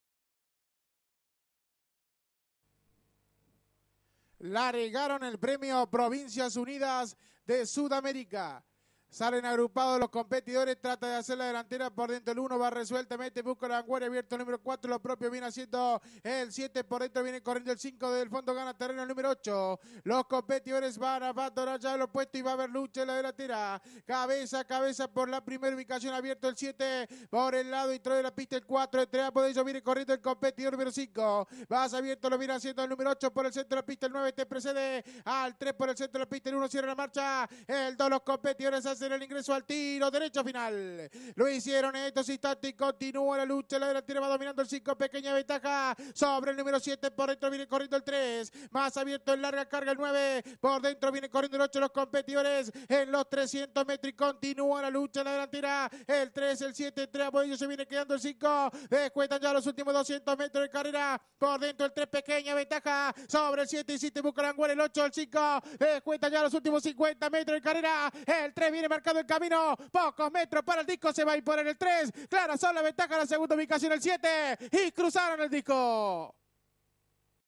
Hipódromo La Punta 05/07/09 - 1ra. Carrera PREMIO PROVINCIAS UNIDAS DE SUD AMERICA - 1100 mts.
Hipódromo La Punta, San Luis, Argentina - Reunión 05/07/09 - 1ra. Carrera PREMIO "PROVINCIAS UNIDAS DE SUD AMERICA - 1100 mts.  Dirección Web
Ganador: 3 ROSA SOUL Campanello, Jose Luis